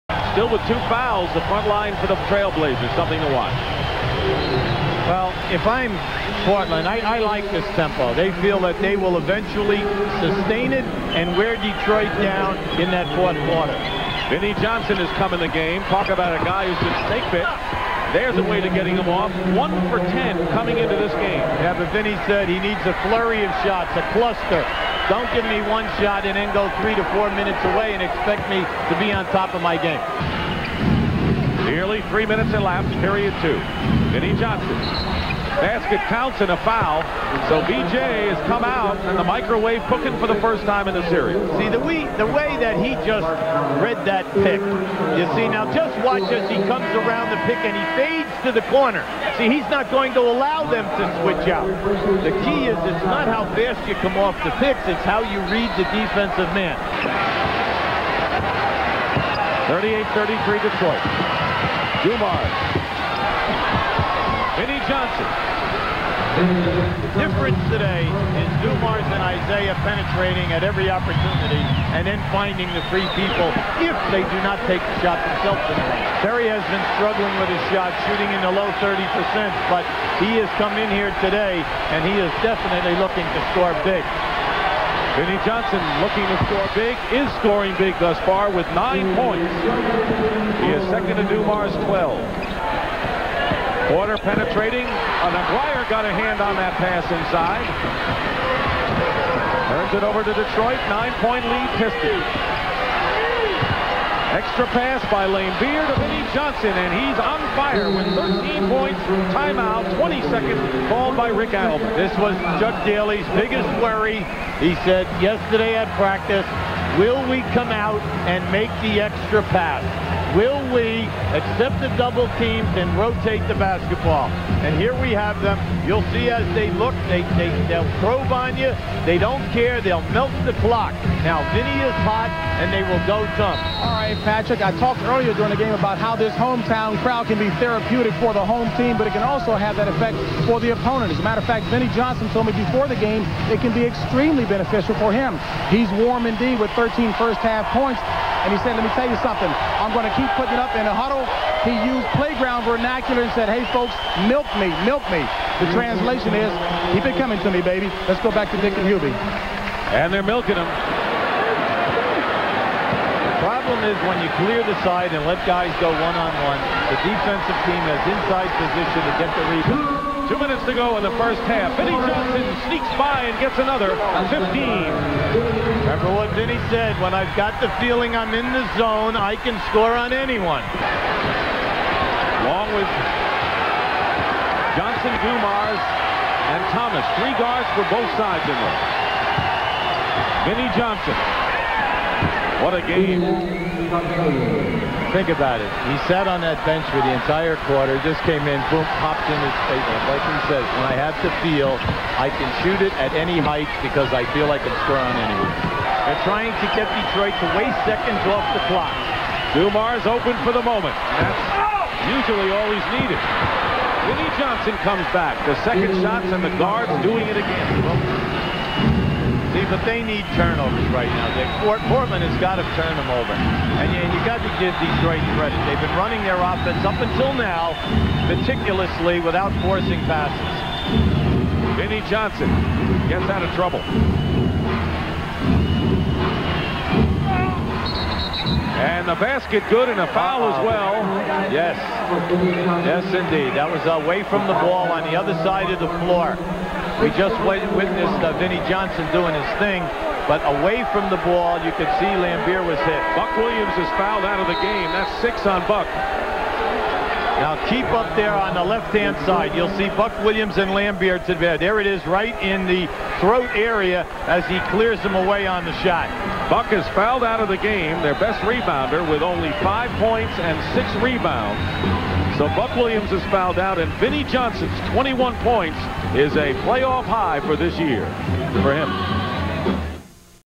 Vinnie Johnson Catches Fire, Scores Finals Career-High (Game 3, 1990)
June 10, 1990 - Nothing better represented the nature of the Microwave like Vinnie Johnson's seven game stretch beginning near the tail end of the 1990 Eastern Conference Finals on through the start of his team's championship series with the Portland Trail Blazers. In the final two games against the Chicago Bulls and first two games against Portland, Johnson averaged just 2.5 points on 12 percent shooting (he was 3-of-25 in those games, including 0-of-10 in Game 6 against Chicago).

Appealing for more offensive opportunities than the four or five shots a night he'd been recently afforded, Johnson sprung to life in Game 3 of the Finals, connecting on 9-of-13 shots for a Finals career-high 21 points in a 15-point Pistons win.

"It's difficult when you come off the bench and you get four shots, six shots," Johnson said afterward. "I'm a scorer, I'm not a shooter. So I need more time, I need more shots for me to be effective some nights. Some nights I can come off and hit the shots right away, some nights it takes a little more time. But once I get to my rhythm, everything's fine."

Johnson's Game 3 performance served as the start a three-game Finals-clinching stretch in which he averaged 19 points on 67 percent shooting, capping off his run with championship-winning jumper in the finals seconds of Game 5.